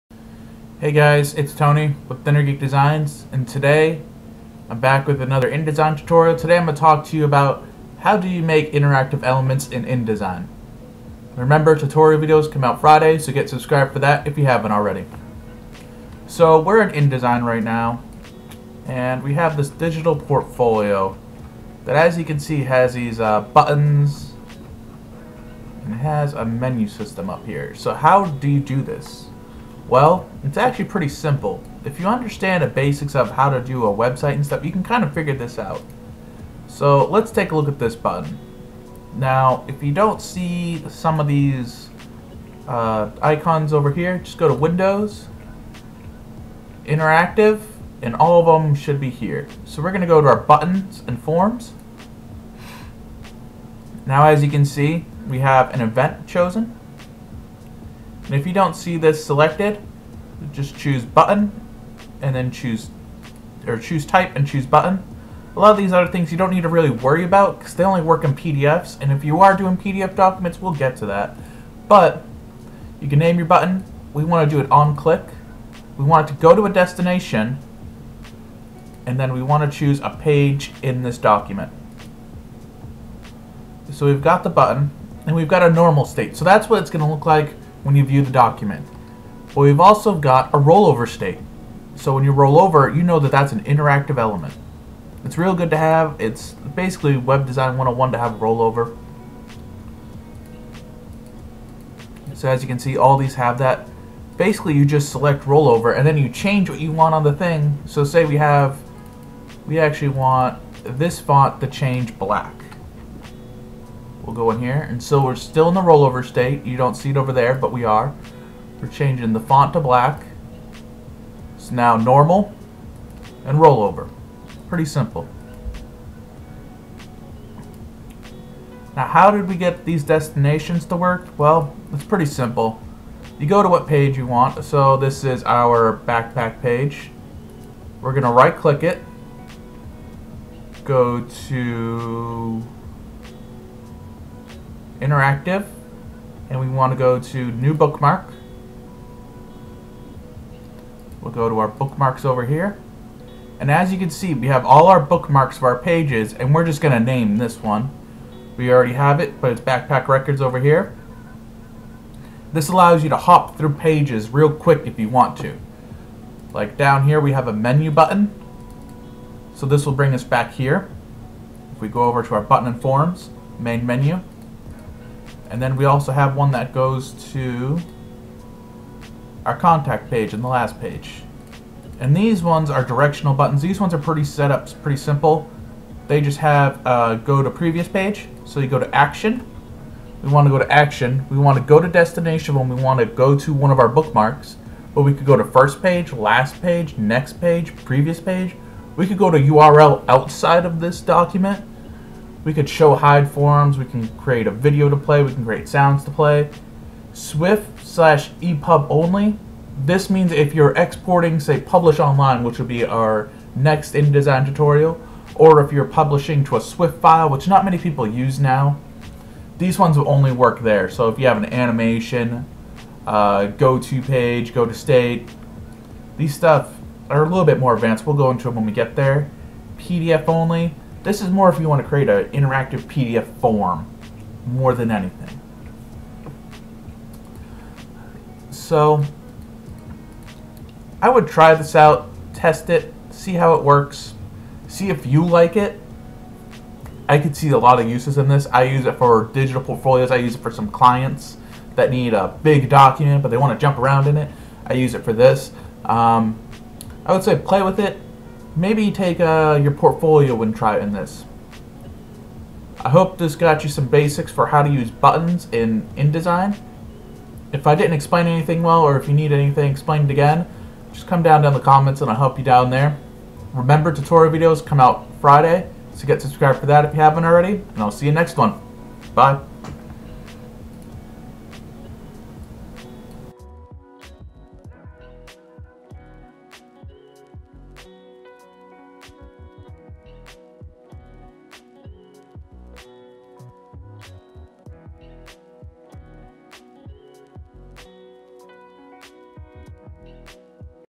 Inner Geek Tutorial Adobe InDesign CC 2017 Add and Edit Interactive Buttons
When it comes to Adobe InDesign CC you do have the ability to create interactive documents. The best way to show people how to interact with your documents is with interactive buttons. This tutorial will show you how to add and edit interactive buttons in Adobe InDesign CC 2017.